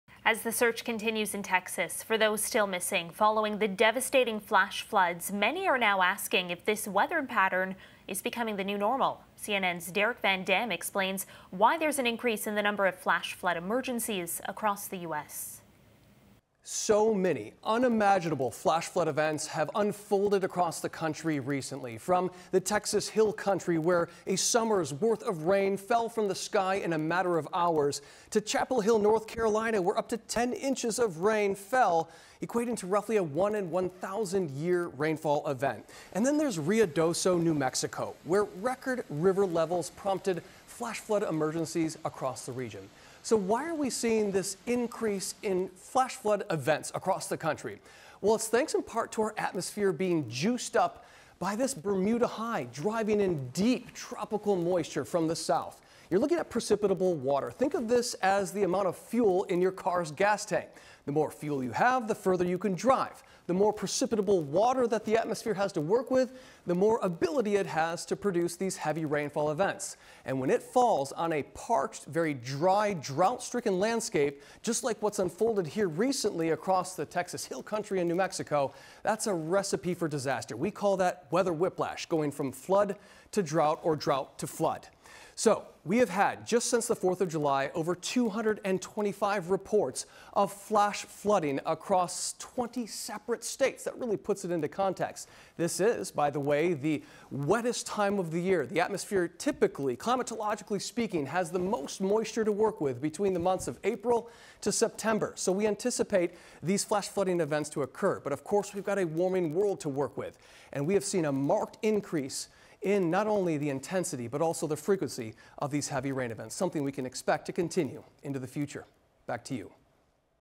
What's behind the uptick in flash flooding events in the U.S.?
CNN's Derek Van Dam explains why the U.S. is reporting more frequent flash flooding events and what it could mean for residents living in vulnerable regions in the future.